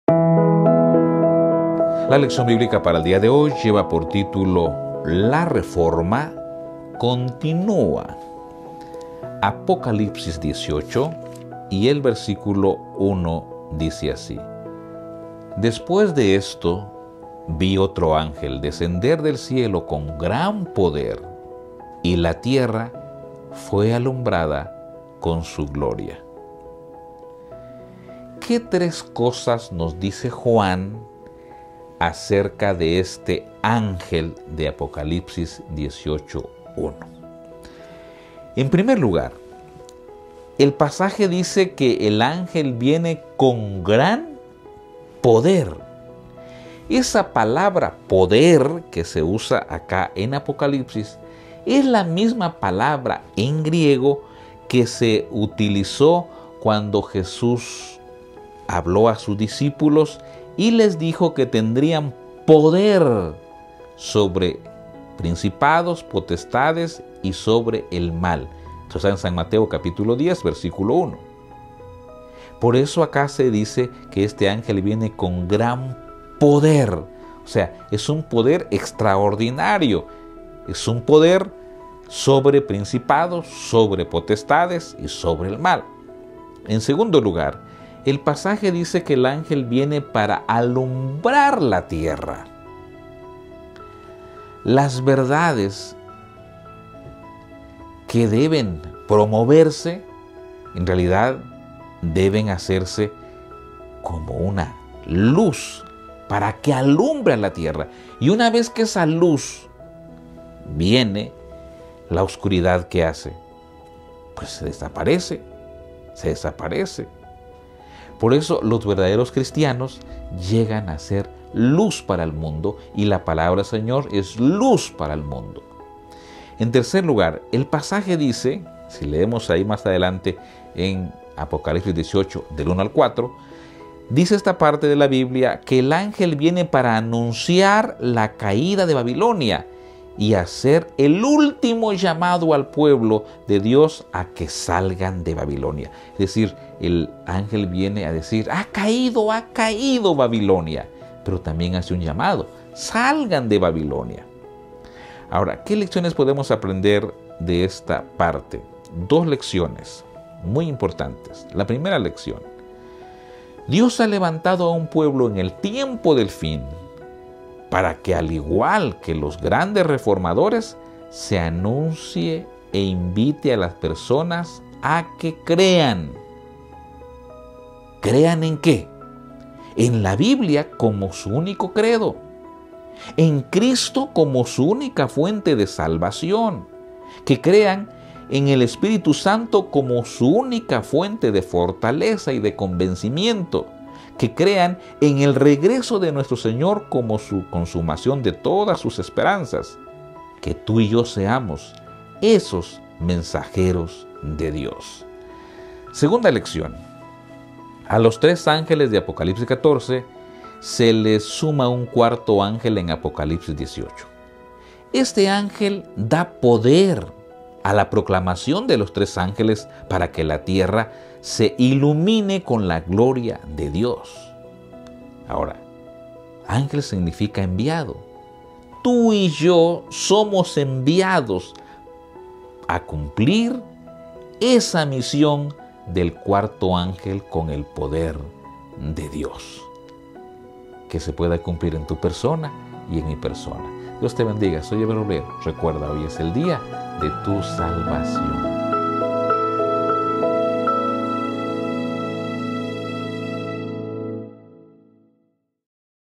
LDB - LA REFORMA CONTINÚA - 20 DE JUNIO 2023
La lección para este día se titula: LA REFORMA CONTINÚA, un tema presentado por el Pastor Ever Roblero.